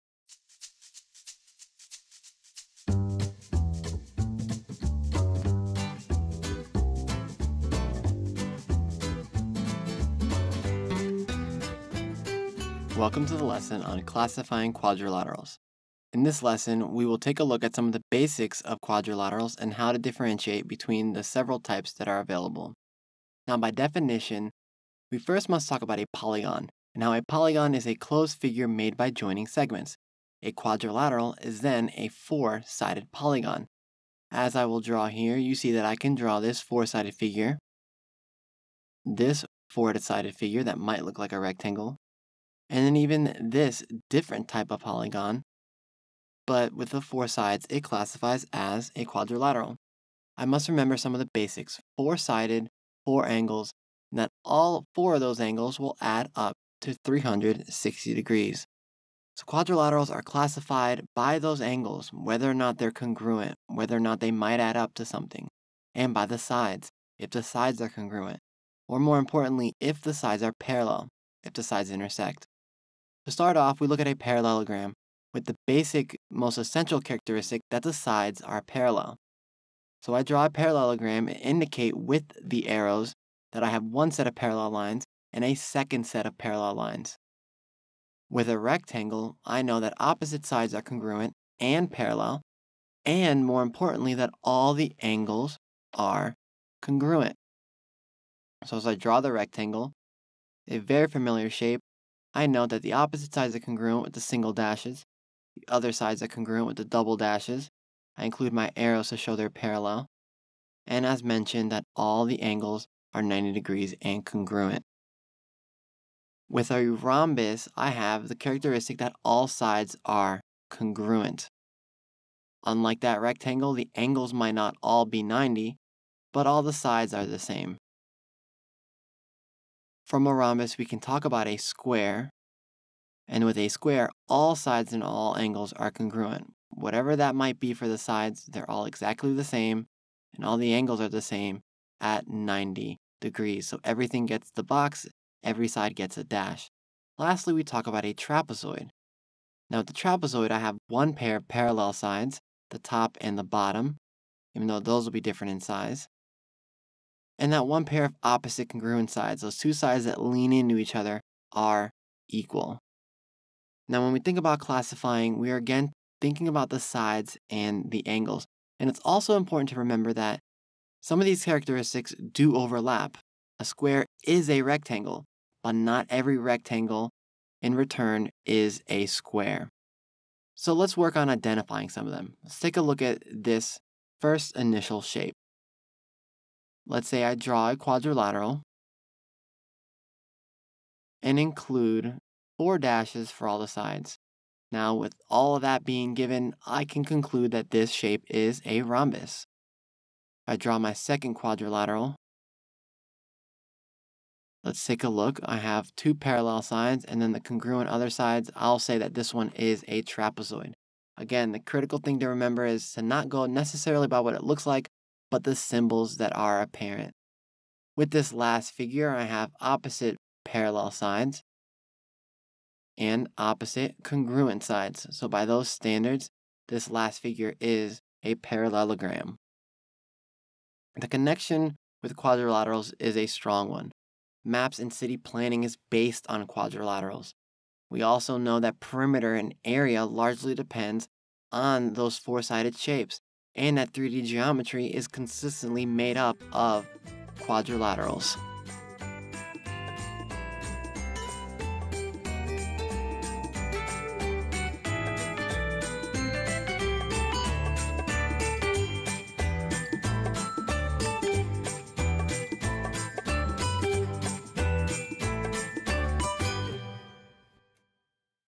Classifying Quadrilaterals
In this lesson we will learn some basics of quadrilaterals and  how to differentiate between them.